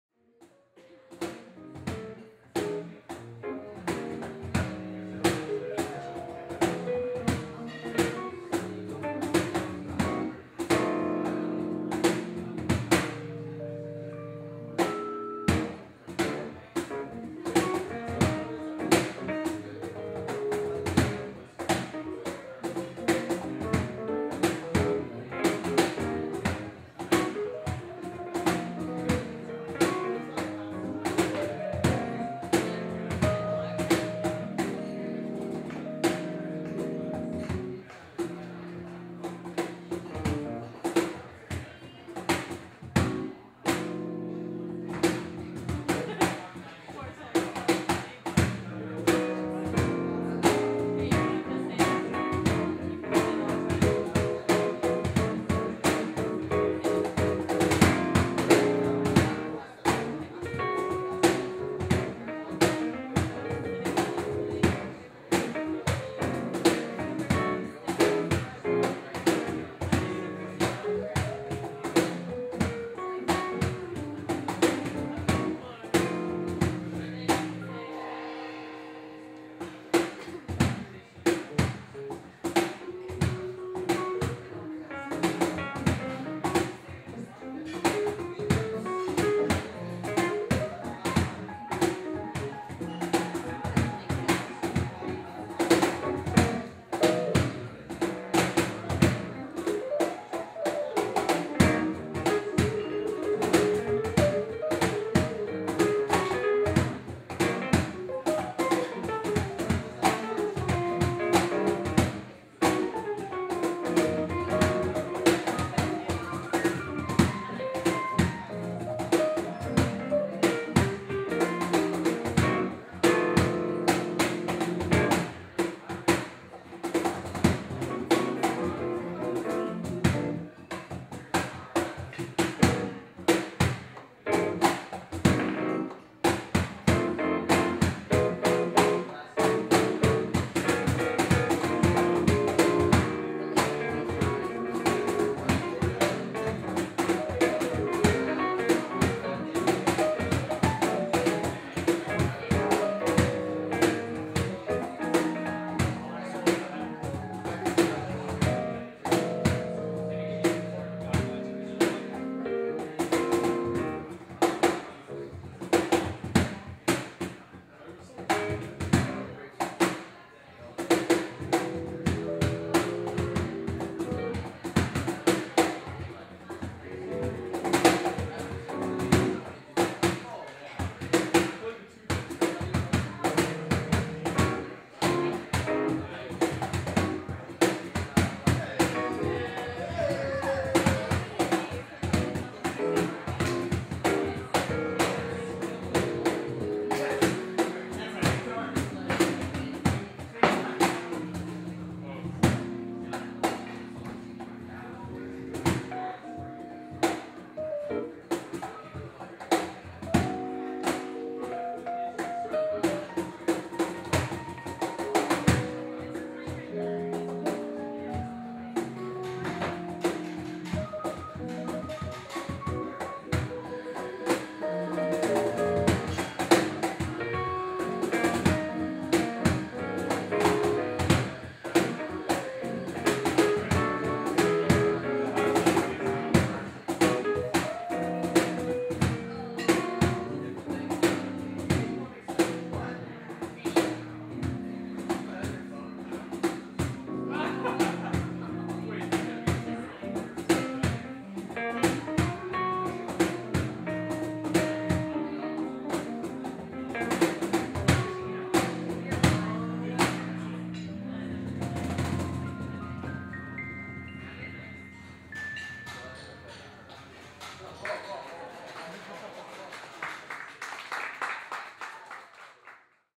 Caswell VS Pruden Round 2 - Battle 2
Caleb Caswell and Chris Pruden battle it out at Cafe Haven Thursday evening, August 5th. If you're in the Sherwood Park area come out every Thursday at 7pm for live music!